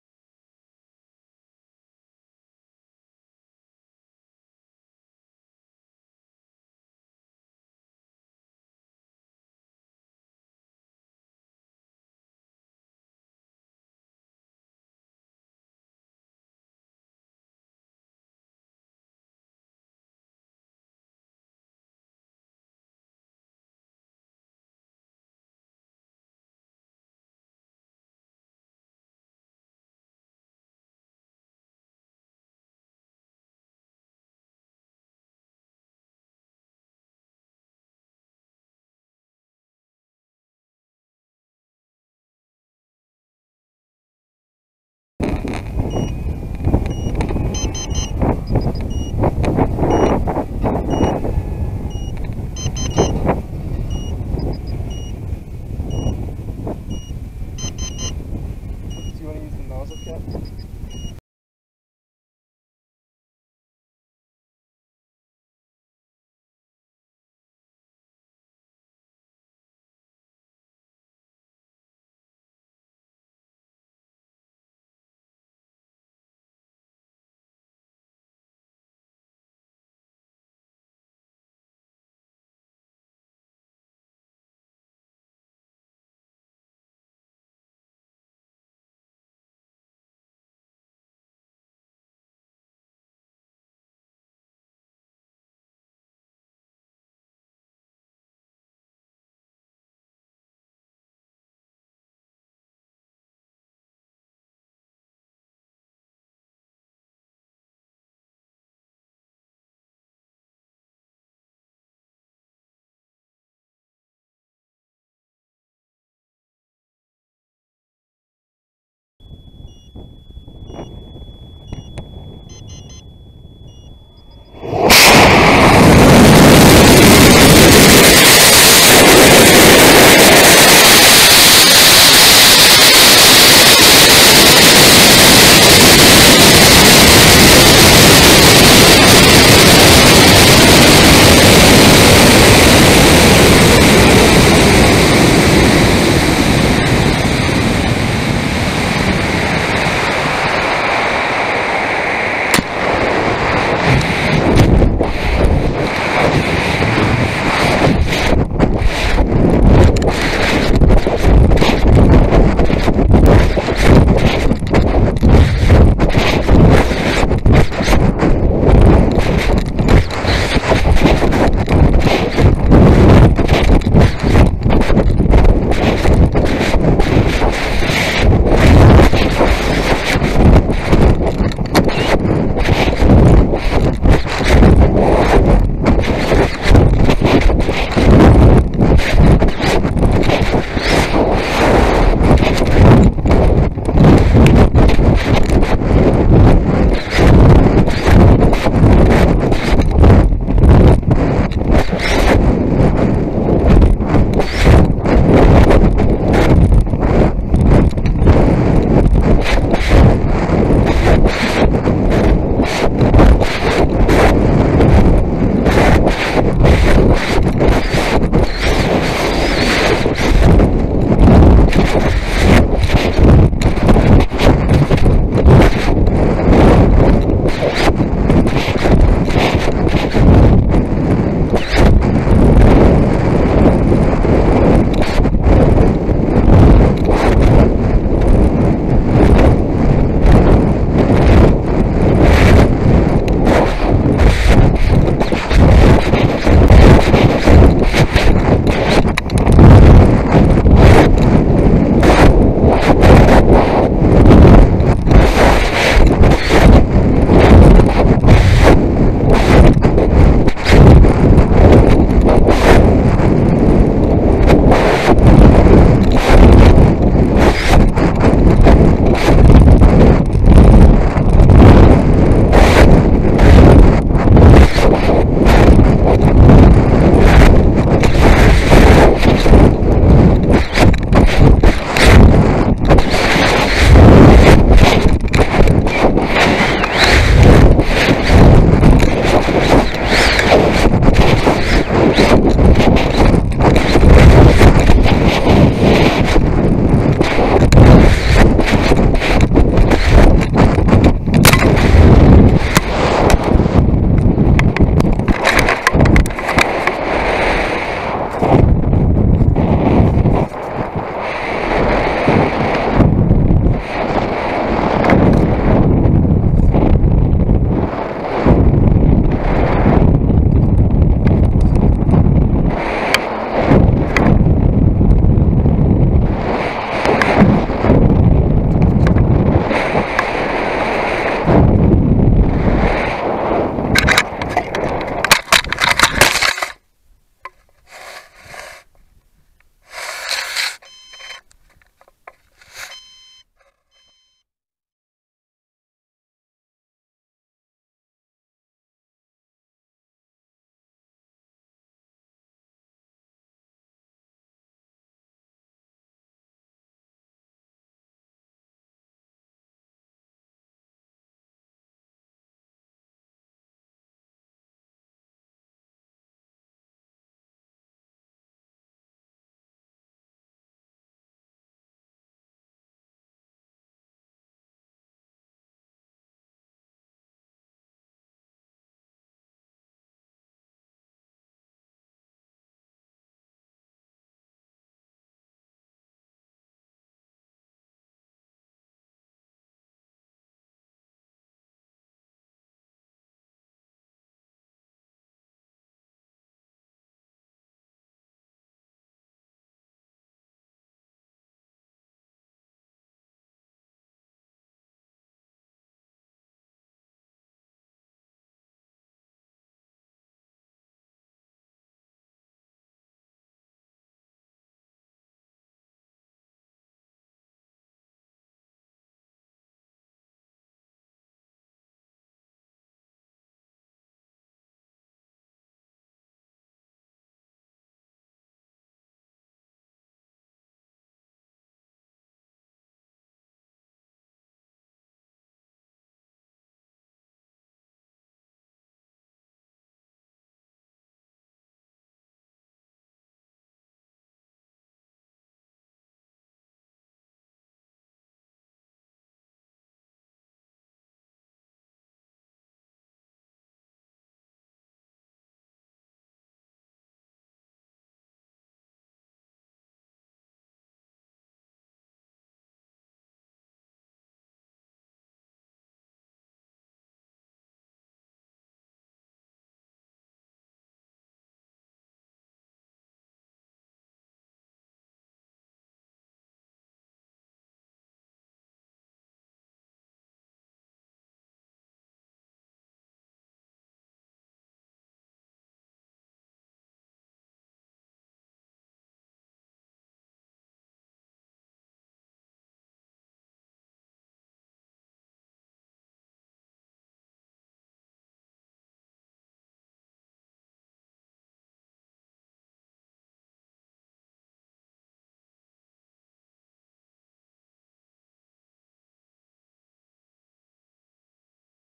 Tripoli L3 - Californication - M1315W
Video from my successful Tripoli L3 Certification flight on May 24th, 2020. The rocket is a 2/3rd Scale Astrobee D called Californication flying on an Aerotech M1315W. Apogee was 13,052ft with a max speed of 850 mph.

Drone footage courtesy of Kelsey Black